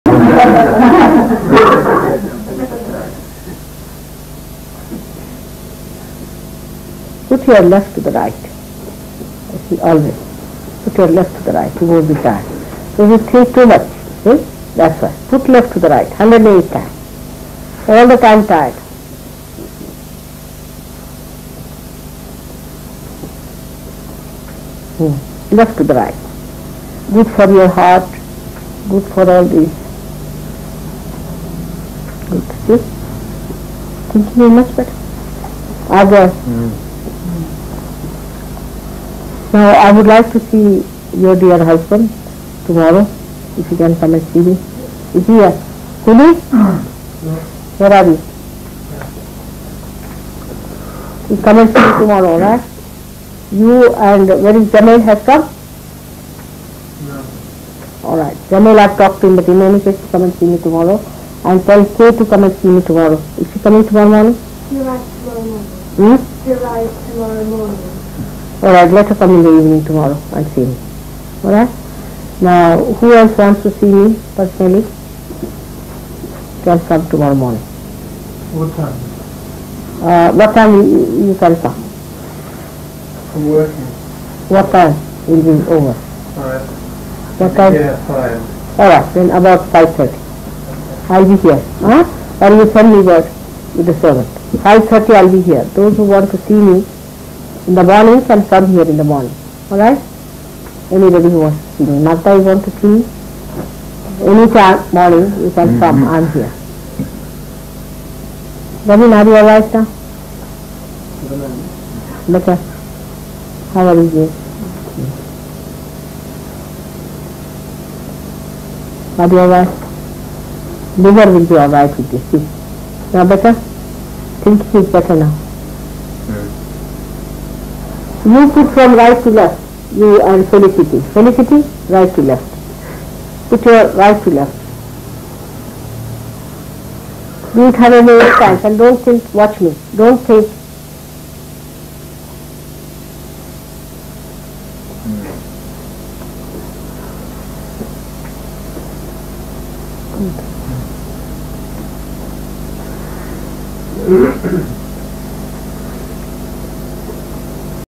1981-10-27 e5: left to the right
...Put your left to the right. I see always. Put your left to the right. Two more every time. So, you think too much. Hmm? That's right. Put left to the right. Hundred and eight time. All the time tired.
Hmm. Left to the right. Good for your heart, good for all this...
good, see? Think you are much better.
Liver will be all right with this. See? Feel better? Think feels better now.
You should from right to left, you and Felicity. Felicity? Right to left. Put your right to left.
One hundred and eight times, and don't think, watch me. Don't think.
Good.